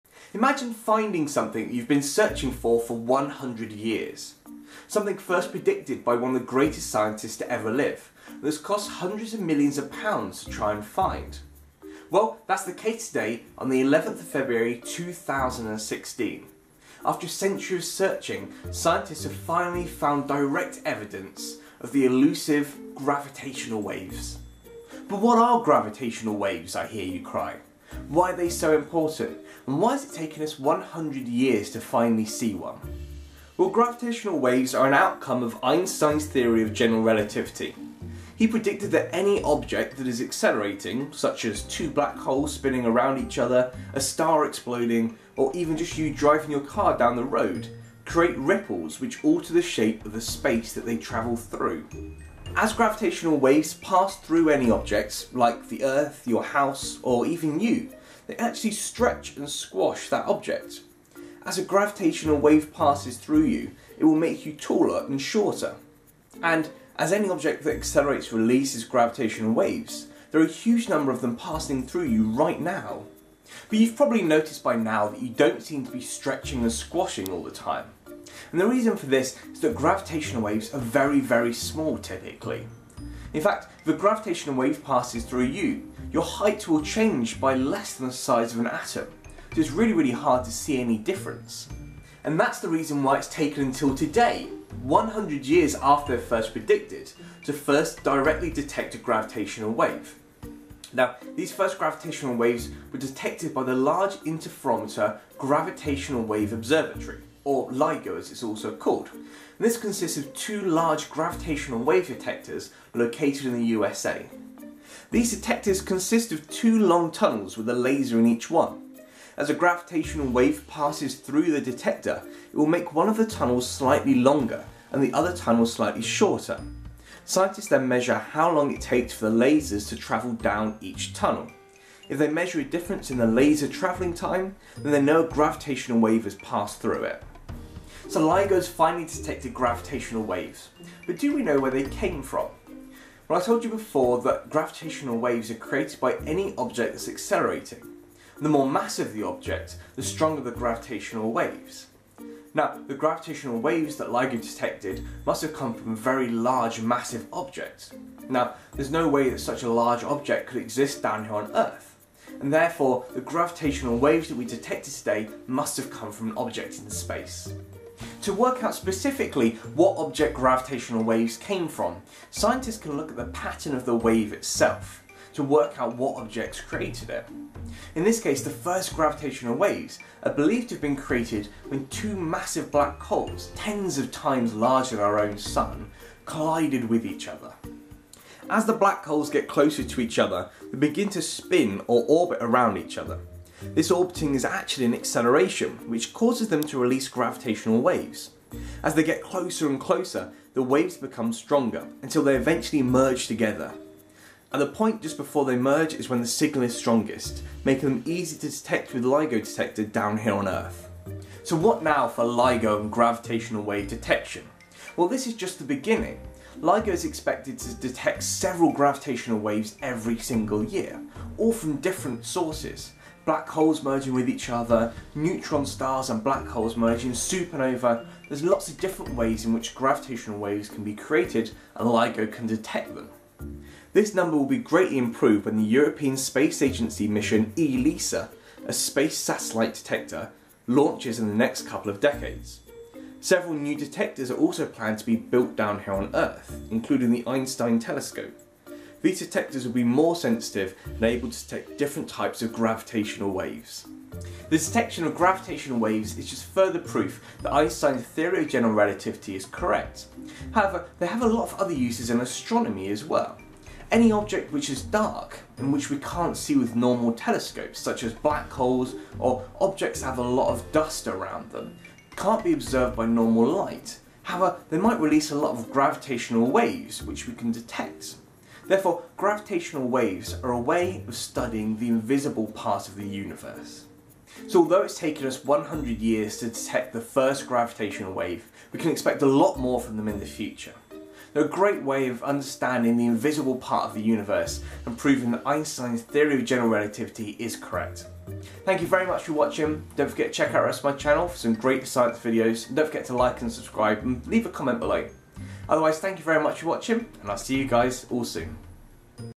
The First Detection of Gravitational Waves
After a century of searching, we have finally directly detected the first gravitational wave. What are Gravitational Waves, what caused these first detections and why are they important.

Credits:
credit is listed to NASA., Public Domain,
An artistic view of the proposed Einstein Telescope observatory. Credit: ASPERA
By NASA/Apollo 17 crew; taken by either Harrison Schmitt or Ron Evans (direct link), Public Domain,